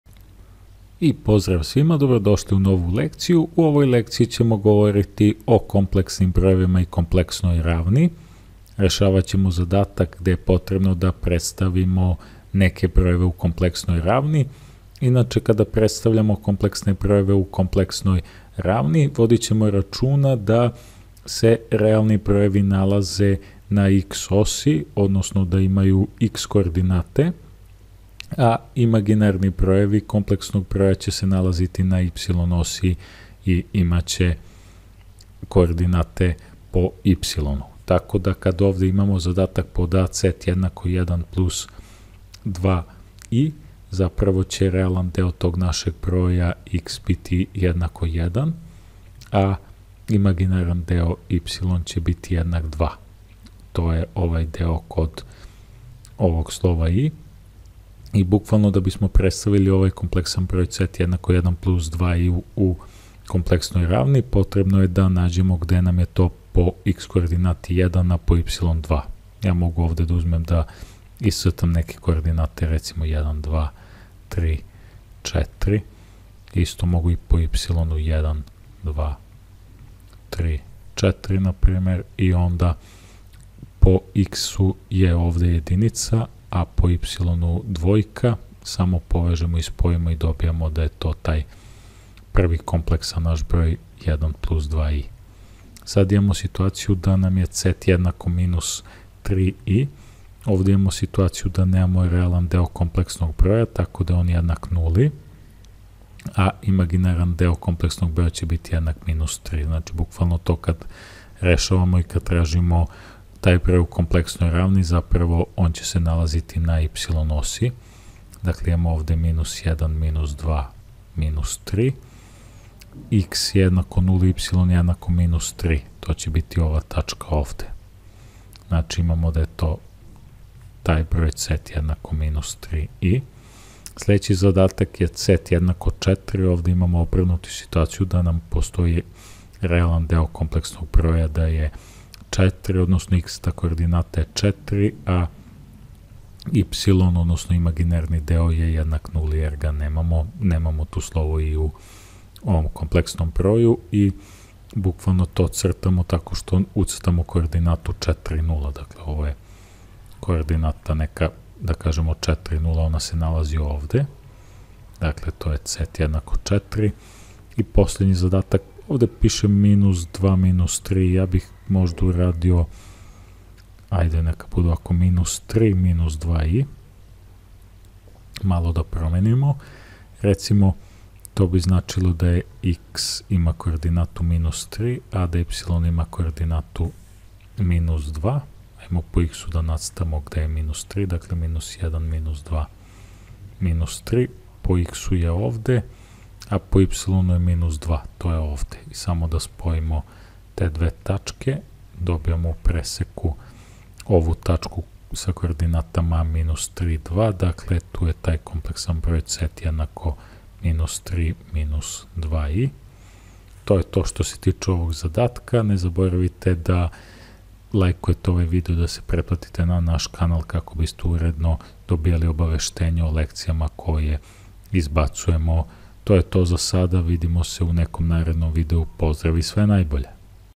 Kompleksni brojevi i kompleksna ravan
Ovo je lekcija na temu kompleksni brojevi i kompleksna ravan. Kompleksne brojeve predstavljamo u kompleksnoj ravni. Matematika za 2 razred srednje skole.